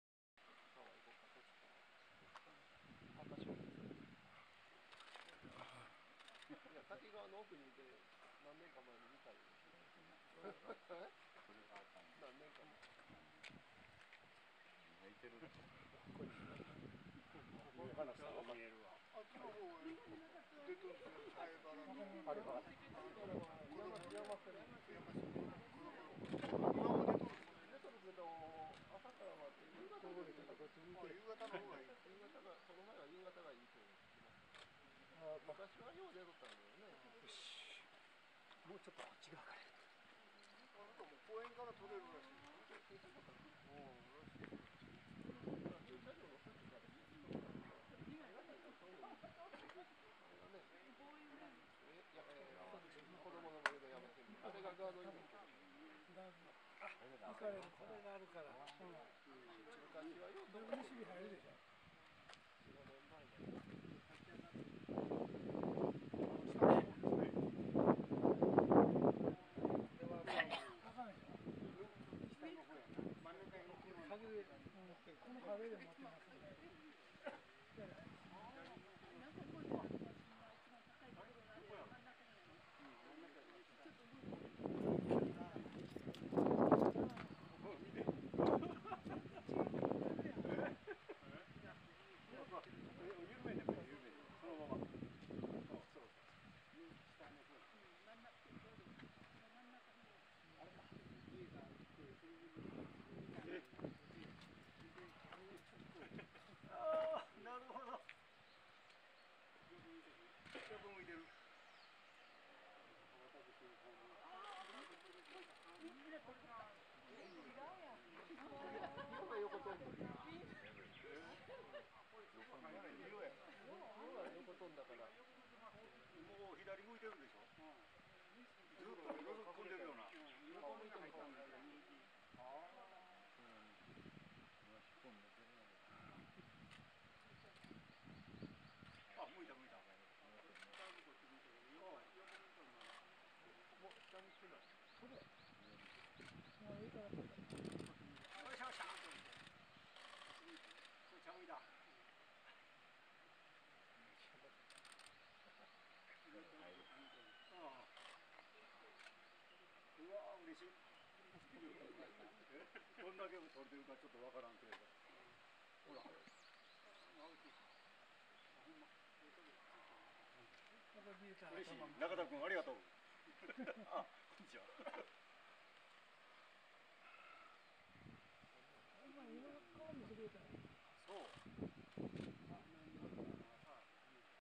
ふくろう 親鳥 2014年5月16日
山の方からヒナを見張っていました。

TSN-884 + TE-11WZ + Nikon1 V3 18.5mm/f1.8